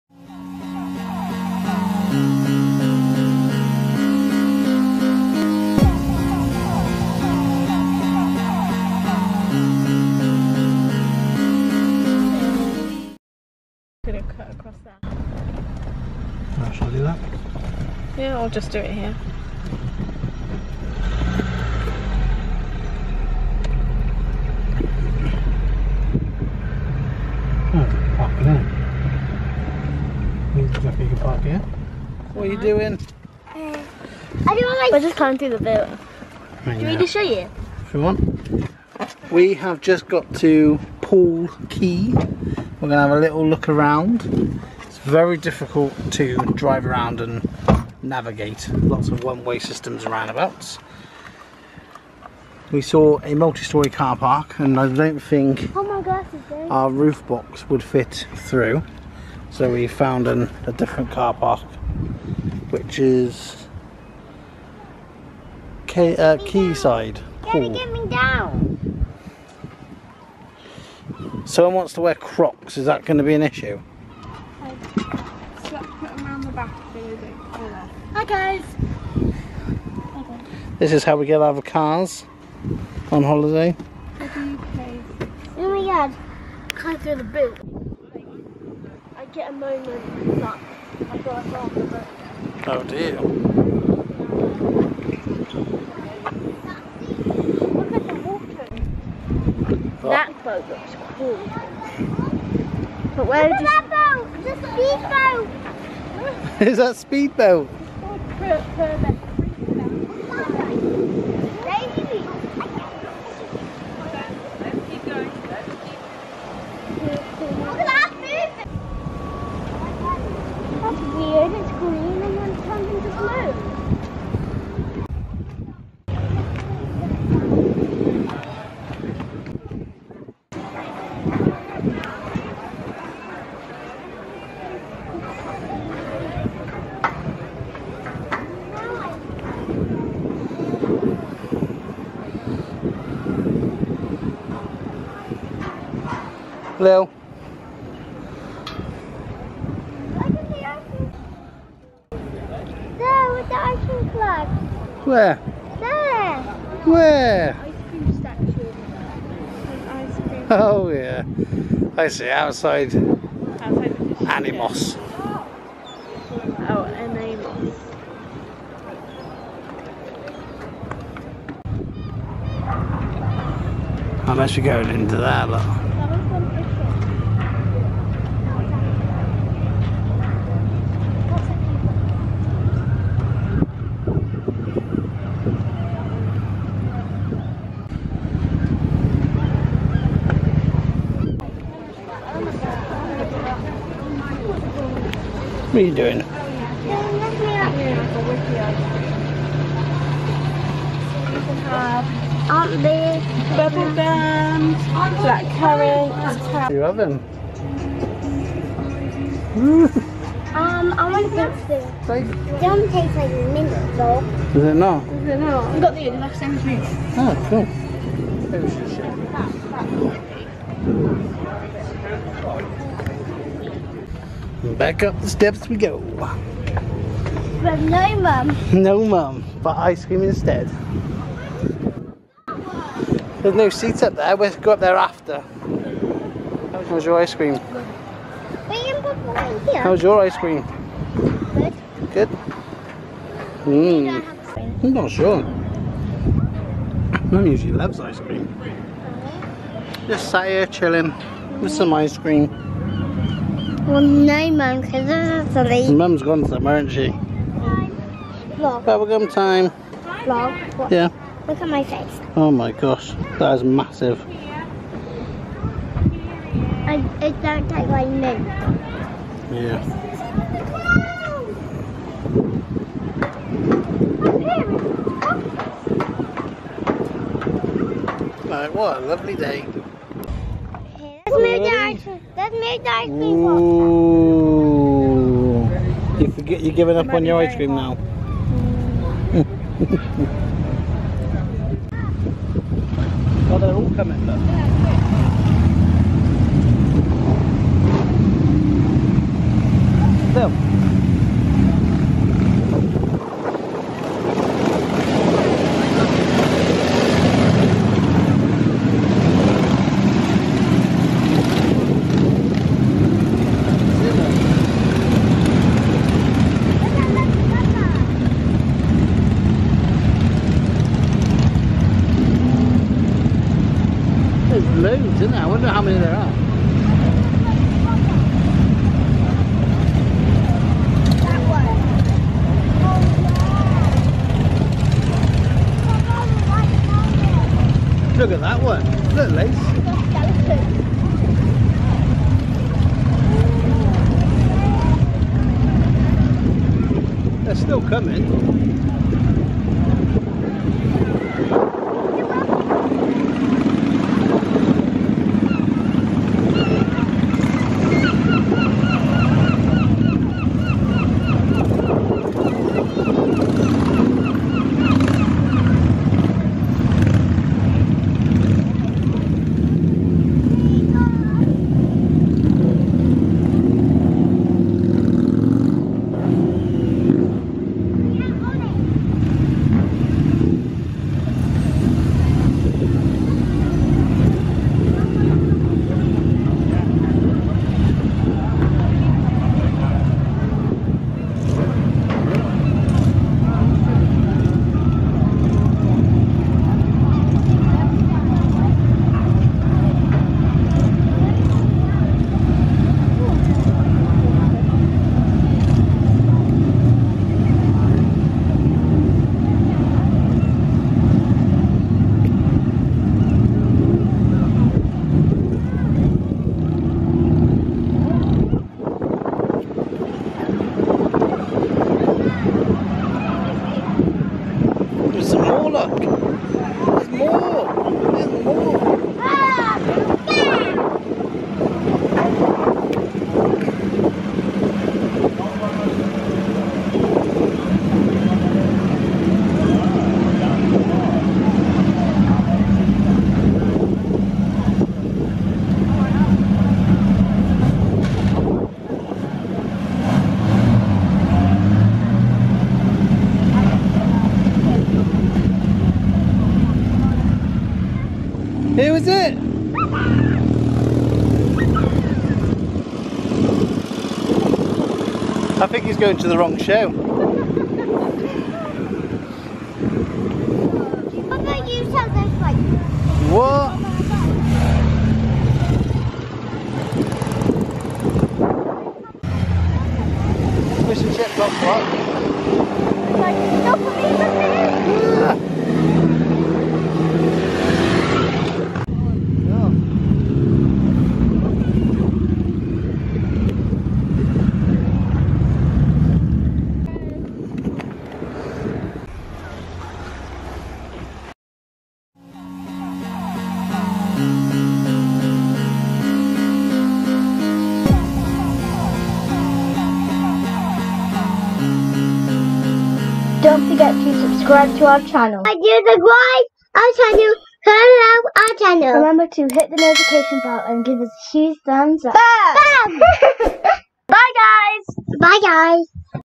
Poole Quay Visit | Dream Machines Motorcycle Event Poole Quay Harbour | Fish and Chips in Poole Quay
A Little visit to Poole Quay, which is up the road from Rockley Park Haven. Have the BEST Fish and Chips from

When we arrived we also noticed a Motorcycle Event was taking place called Dream Machines.

We got to see some amazing bikes driving through the harbour.

Poole Quay Visit | Bike Meet in Poole Quay Harbour | Fish and Chips in Poole Quay | Poole Harbour